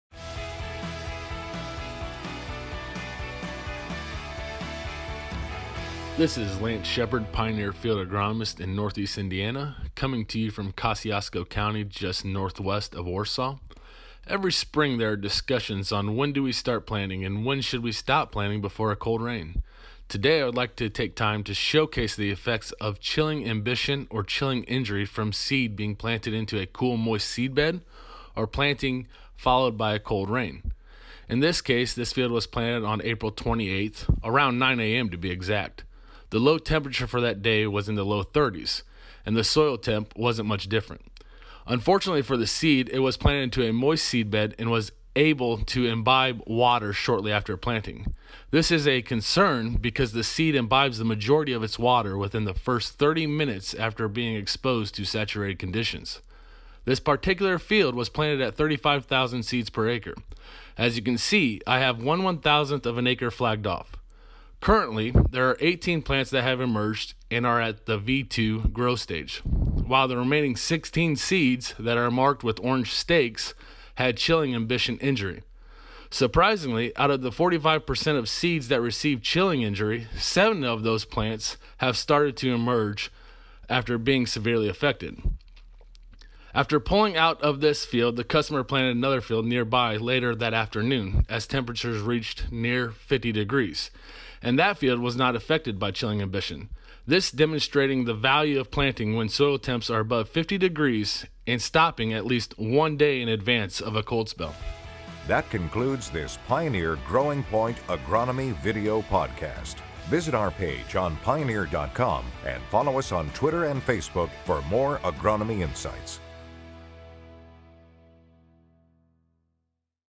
When to Stop Planting Before a Cold Rain
Pioneer Field Agronomist Lance Shepherd talks about what can happen when seeds suffer chilling imbibition and when you should plant to avoid such an injury.